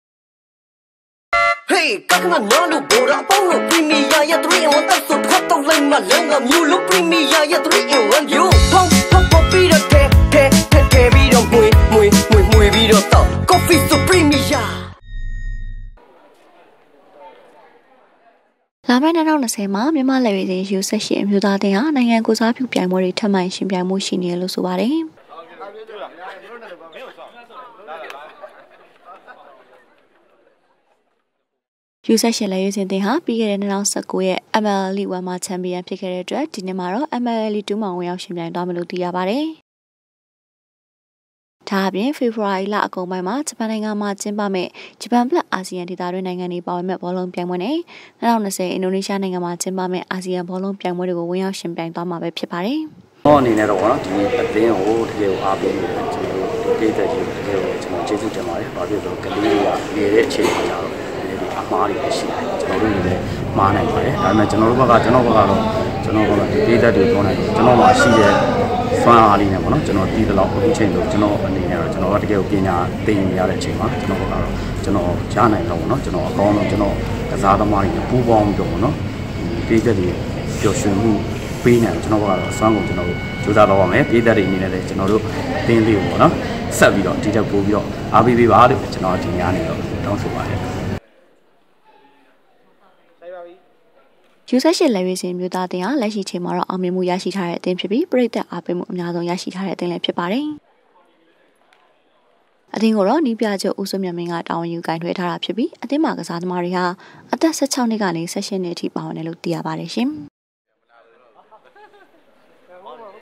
လာမယ့်နှစ်အတွင်းနိုင်ငံတကာ ပြိုင်ပွဲတွေ ဝင်ပြိုင်မယ့် မြန်မာယူ ၁၈
လာမယ့် ၂၀၂၀ မှာတော့ မြန်မာ့လက်ရွေးစင် ယူ ၁၈ အမျိုးသားအသင်းဟာ နိုင်ငံကိုယ်စားပြု ပြိုင်ပွဲတွေ ထပ်မံ ယှဉ်ပြိုင်ဖို့ ရှိနေတယ်လို့ ဆိုပါတယ်။

ယူ ၁၈ လက်ရွေးစင် အသင်းဟာ ပြီးခဲ့တဲ့ ၂၀၁၉ ခုနှစ်ရဲ့ MNL League 1 မှာ ချန်ပီယံဖြစ်ခဲ့တဲ့အတွက် ဒီနှစ်မှာတော့ MNL League 2 မှာ ဝင်ရောက်ယှဉ်ပြိုင်သွားမယ်လို့ သိရပါတယ်။

ဒါ့အပြင် ဖေဖော်ဝါရီလအကုန်ပိုင်းမှာ ဂျပန်နိုင်ငံမှာ ကျင်းပမယ့် ဂျပန် + အာဆီယံ ဒေသတွင်း နိုင်ငံ တွေပါဝင် မယ့် ဘောလုံးပြိုင်ပွဲ နဲ့ ၂၀၂၀ အင်ဒိုနီးရှားနိုင်ငံမှာ ကျင်းပမယ့် အာဆီယံ ဘောလုံးပြိုင်ပွဲတွေကို ဝင်ရောက်ယှဉ်ပြိုင်သွားမှာပဲ ဖြစ်ပါတယ်။

ယူ ၁၈ လက်ရွေးစင် အမျိုးသားအသင်းဟာ လက်ရှိအချိန်မှာ အောင်မြင်မှု ရရှိထားတဲ့ အသင်းဖြစ်ပြီး၊ ပရိသတ်အားပေးမှု အများဆုံးရရှိထားတဲ့ အသင်းလည်း ဖြစ်ပါတယ်။

အသင်းကိုတော့ နည်းပြချုပ် ဦးစိုးမြတ်မင်းက တာဝန်ယူ ကိုင်တွယ်ထားတာဖြစ်ပြီး၊ အသင်းမှာ ကစားသမားတွေဟာ အသက် ၁၆ နှစ်ကနေ၊ ၁၈ နှစ်အထိ ပါဝင်တယ်လို့ သိရပါတယ်။